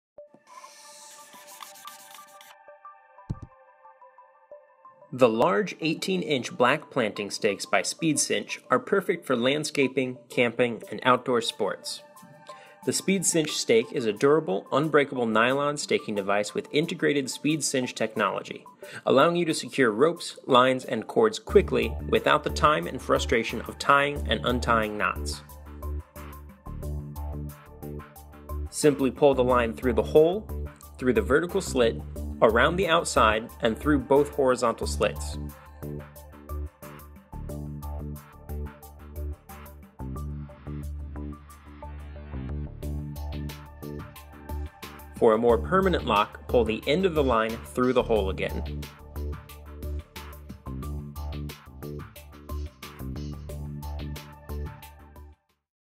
Speed Cinch 18" Stake - TreeStuff.com 360 View
These large 18" black planting stakes by Speed Cinch are perfect for landscaping, camping, and outdoor sports. The Speed Cinch Stake is a durable, unbreakable nylon staking device with integrated Speed Cinch technology, allowing you to secure lines and cords quickly and without the time and frustration of tying and untying knots.